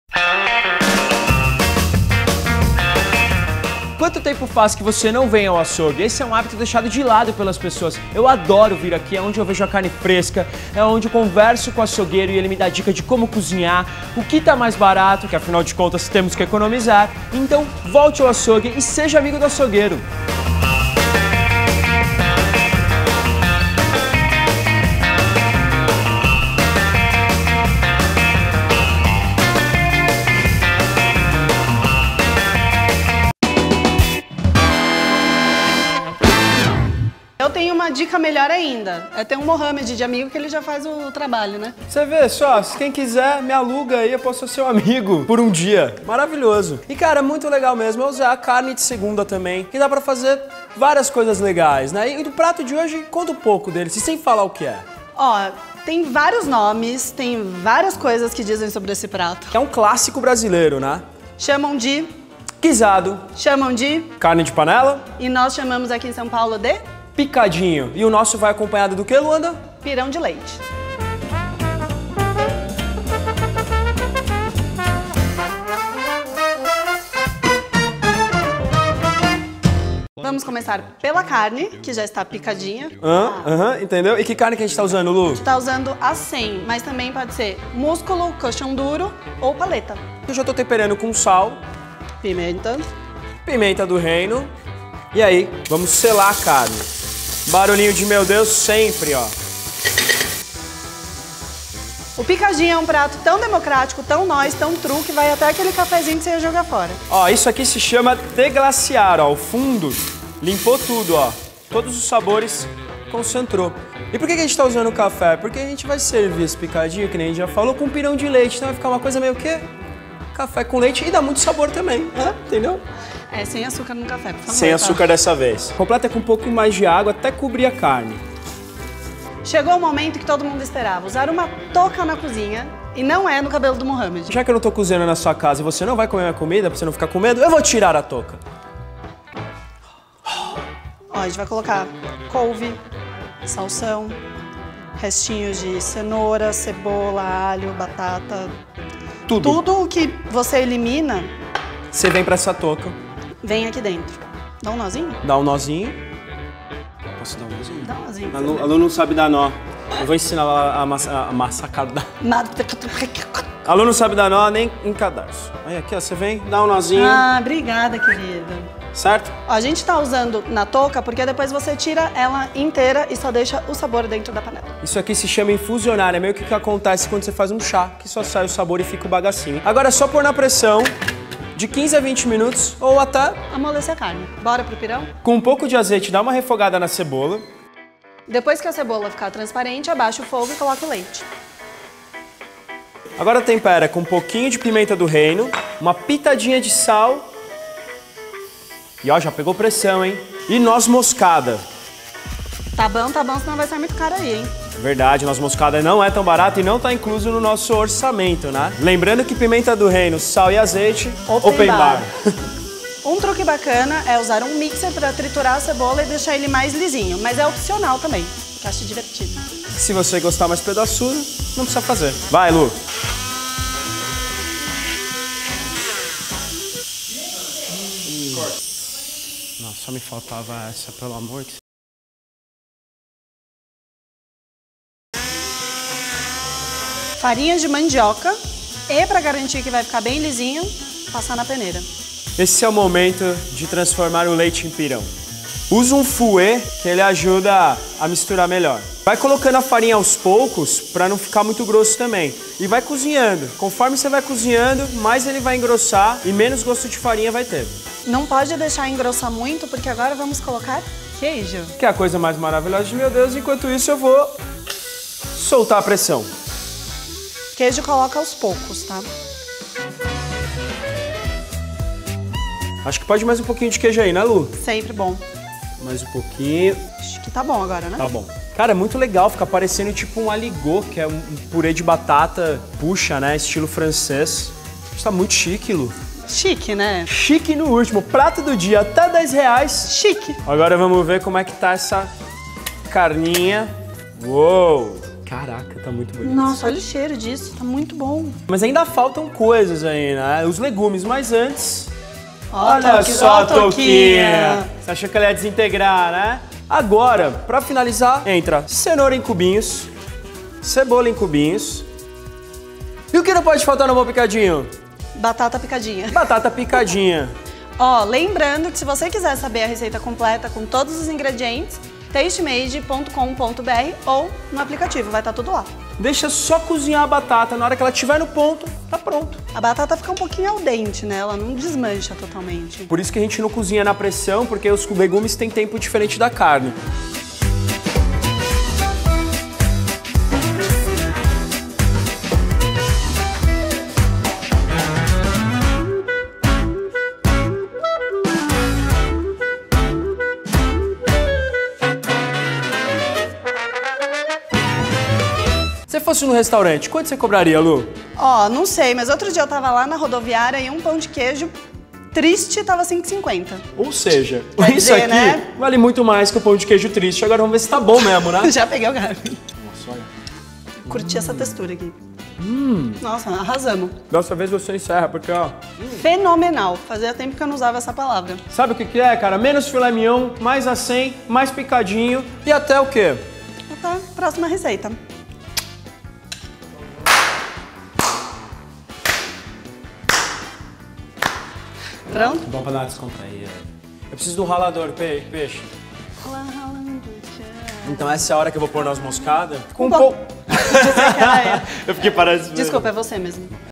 Picadinho com Pirão de Leite | PRATO DO DIA até 10 REAIS
Essa receita é mais um prato especial, lindo e delicioso que a Luanda e o Mohamad preparam nessa temporada: picadinho com pirão de leite!